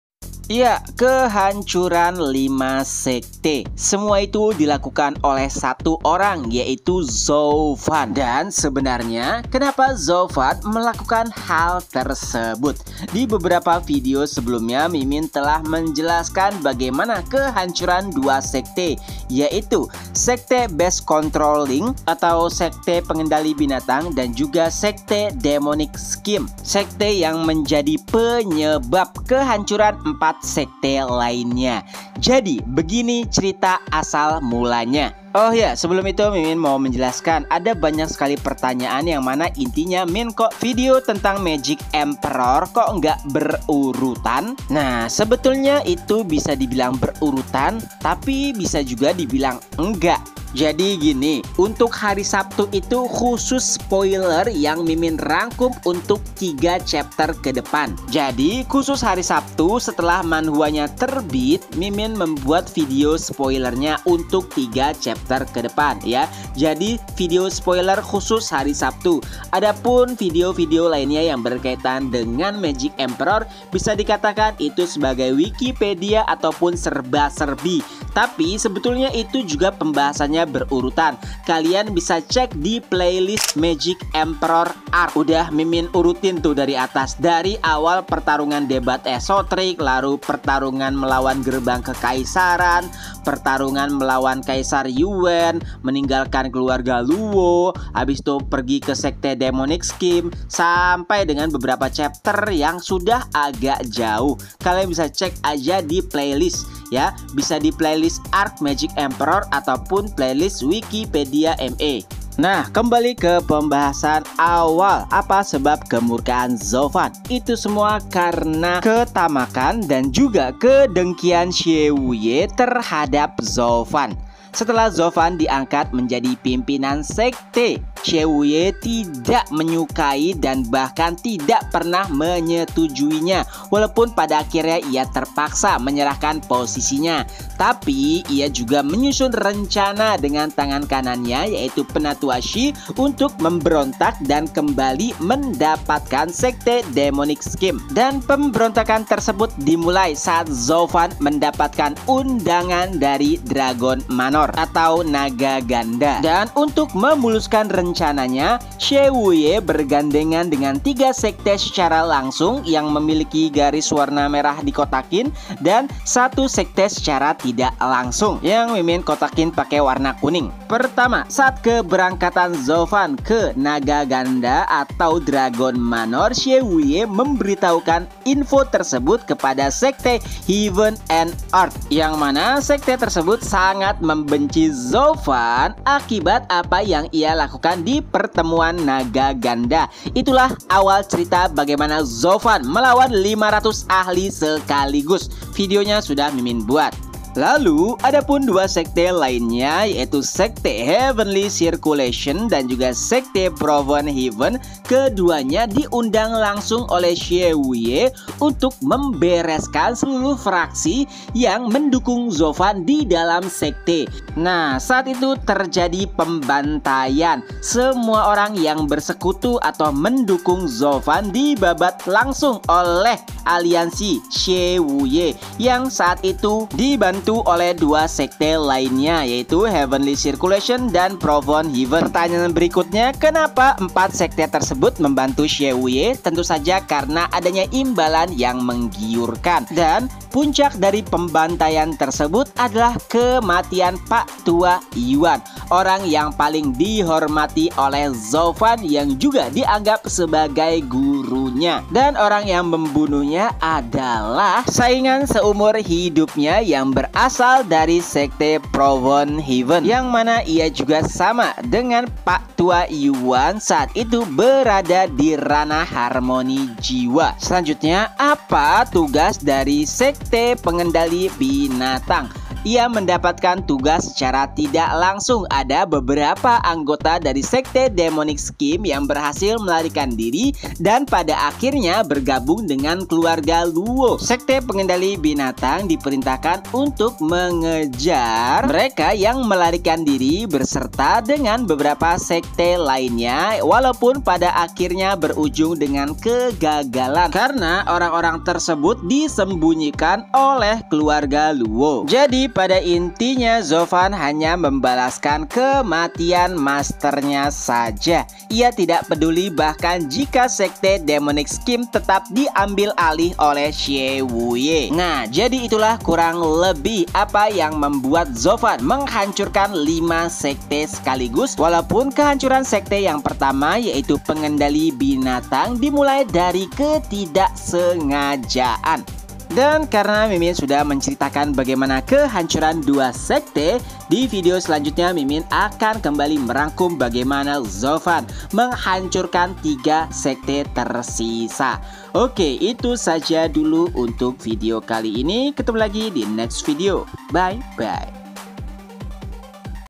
ZHU0 FAN VS 5 SEKTE | AWAL MULA CERITA!
alasan zhu0fan menghancurkan 5 sekte.